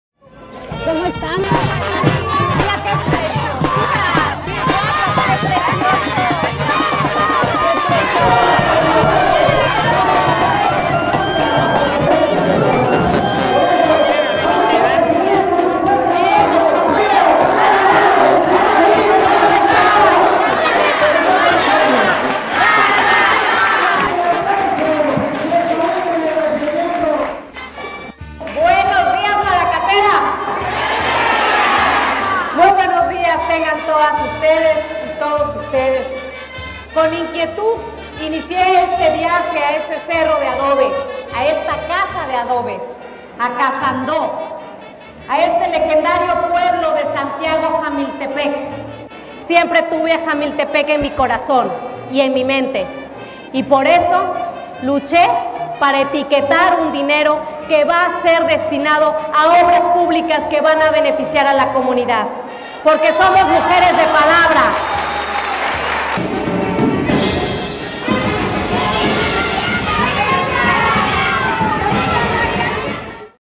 🌺👩🏽Mariana Benítez Tiburcio| Encuentro con Mujeres Santiago Jamiltepec
Hola amigas y amigos, por este medio les comparto mi encuentro con las mujeres de Santiago Jamiltepec.